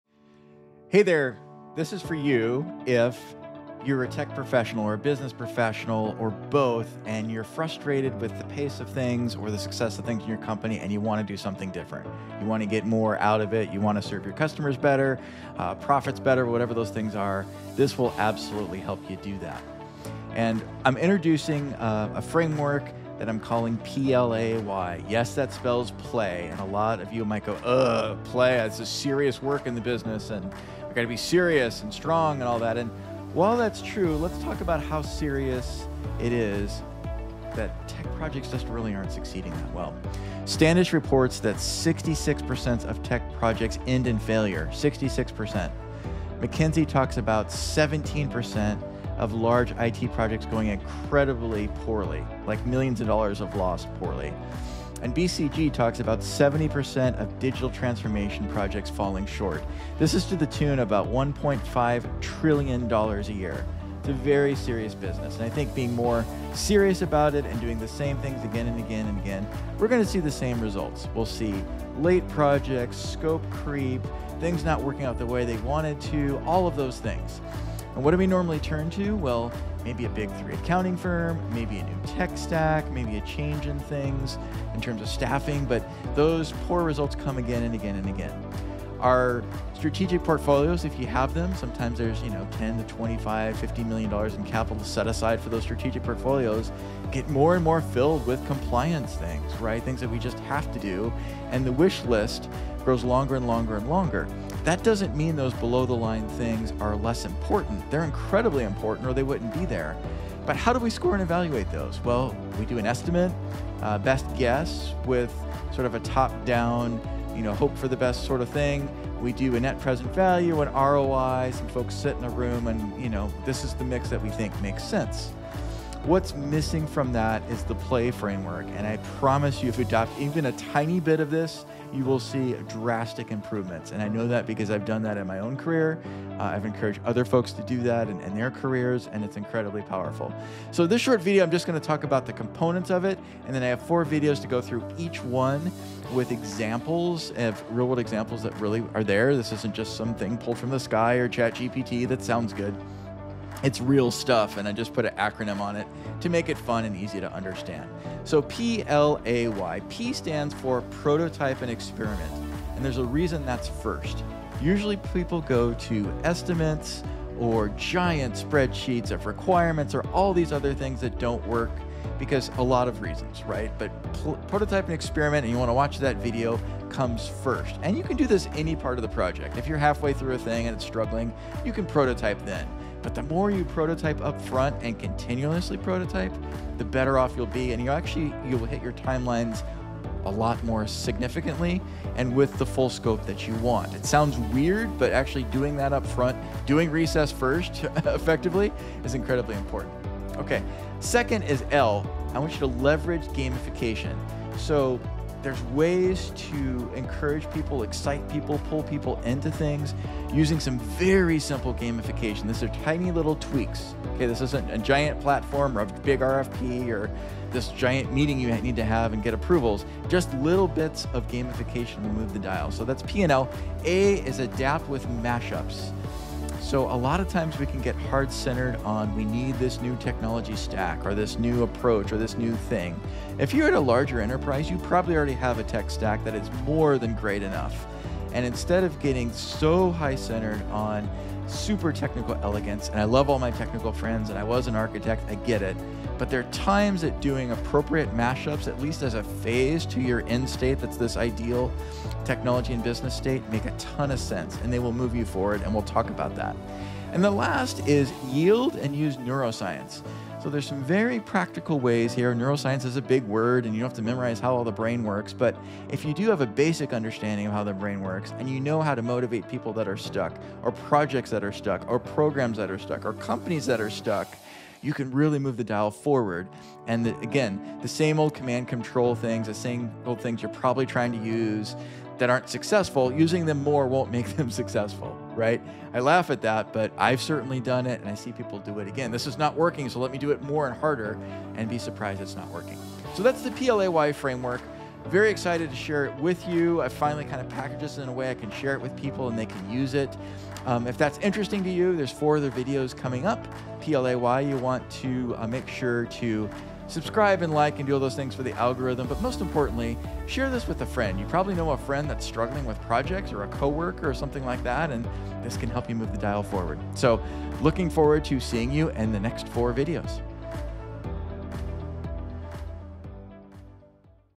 2.4 Trillion Reasons to Solve Tech Project Issues With PLAY
Tired of technical projects running over time, over budget, or not delivering at all? Frustrated by the lack of resources to fund your portfolio projects? Is your project continually not funded, and you feel like you can’t move?
The good news is that there is likely at least $1M in capacity, in what you already have. Even better, there are some simple and proven approaches that can quickly change things for you.

In this first video, we introduce the P.L.A.Y. model  - which helps you get a lot more out of your existing environment, and have a lot more fun doing it.

The four components are:
• Prototype and Experiment
• Leverage Gamification
• Adapt With Mash-Ups
• Yield and Use Neuroscience

3.

Sources
Standish Group – “Approximately 66% of technology projects end in either partial or complete failure, meaning only about 34% are considered successful.”

McKinsey – “17 percent of IT projects go so bad that they can threaten the very existence of the company.”

Boston Consulting Group - “BCG research has found that up to 70% of digital transformations fail to deliver on their objectives.”

CPSQ – “The cost of poor software quality in the US has grown to at least $2.41 trillion.”